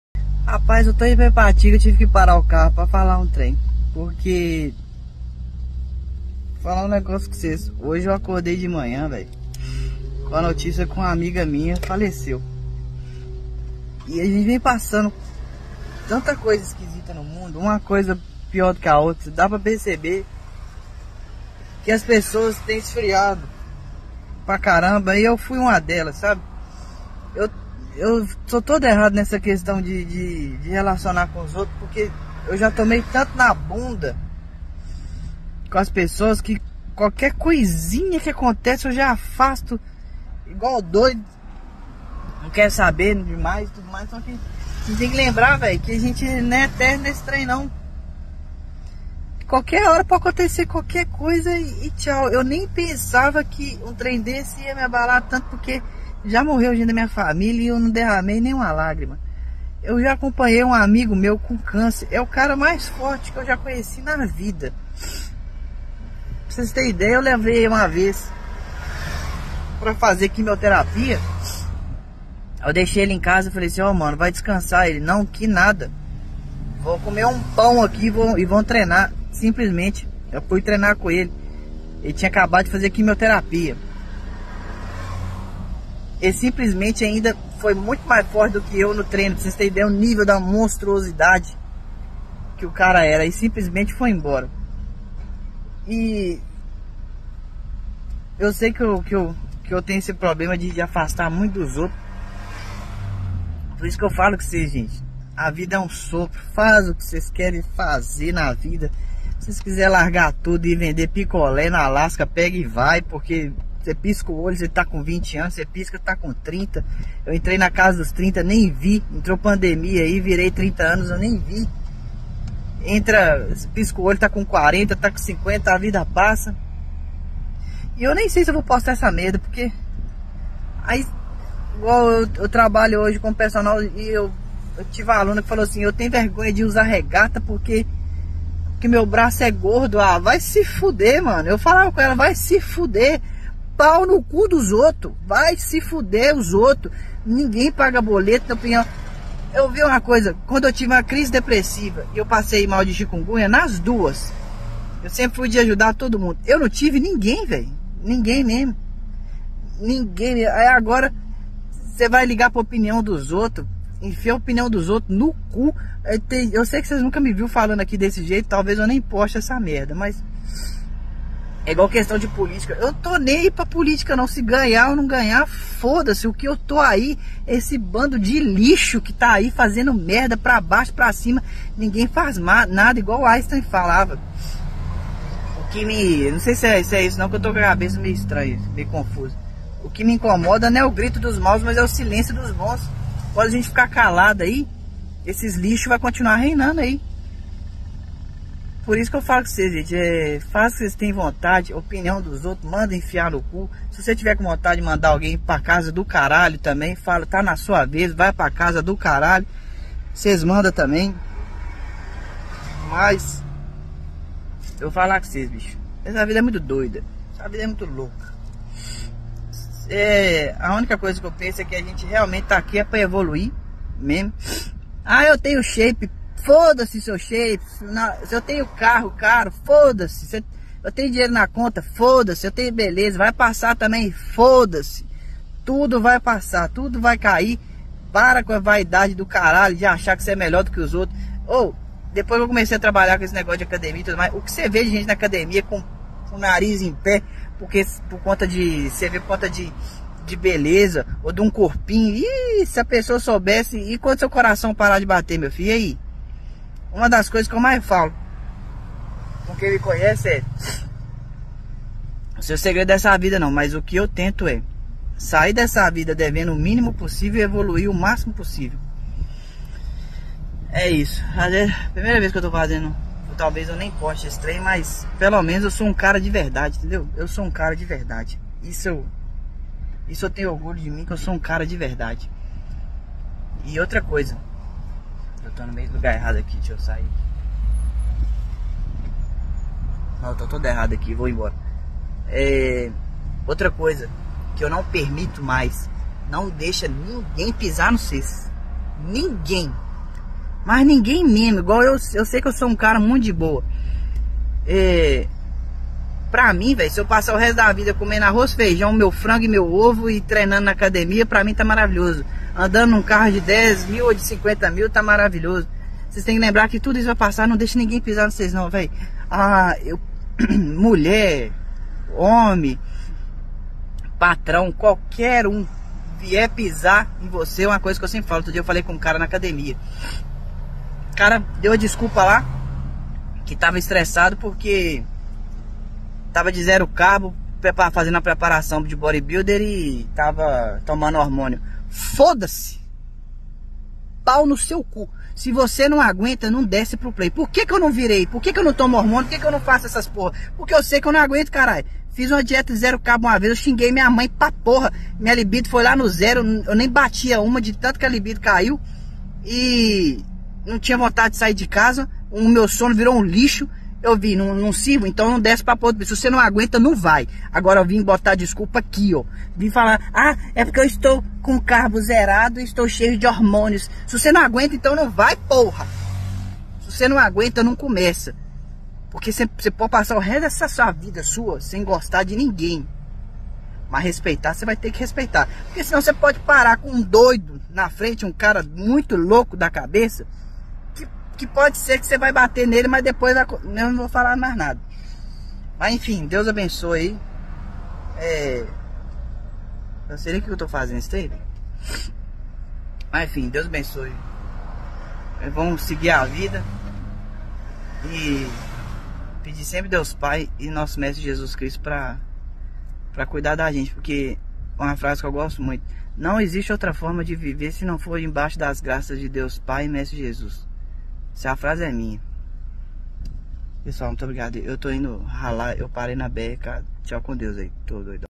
DESABAFO !!! A VIDA E FRAGIL POREM FORTE AO MESMO TEMPO
DESABAFO SOBRE COMO A VIDA E FRAGIL E FORTE AO MESMO TEMPO... insta @euaugustorodrigues